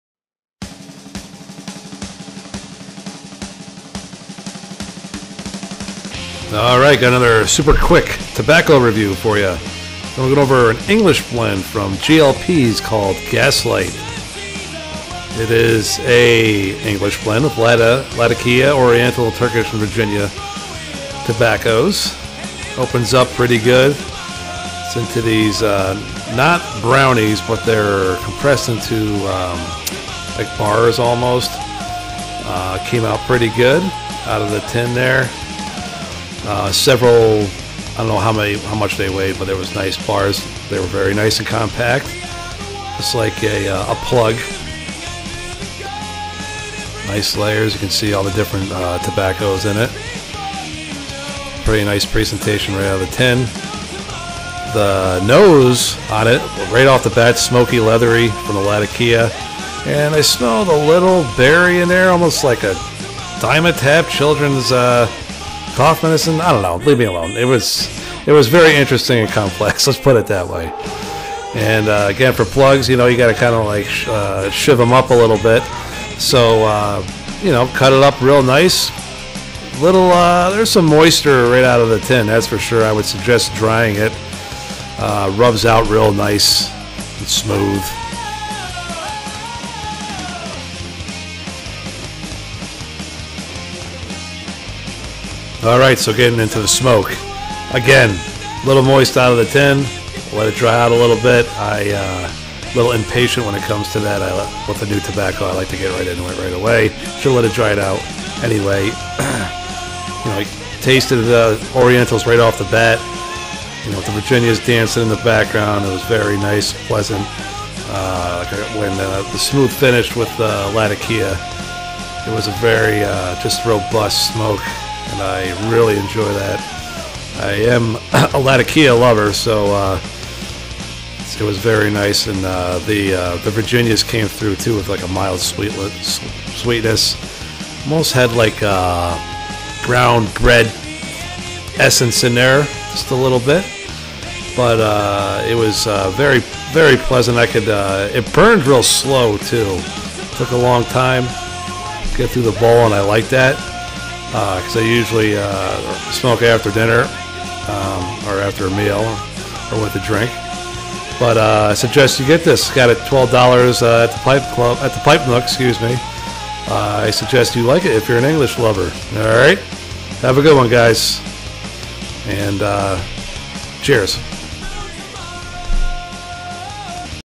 Pipe Tobacco Review **GL Pease** GASLIGHT
Quick review on a newly purchased English Blend. Cheers!!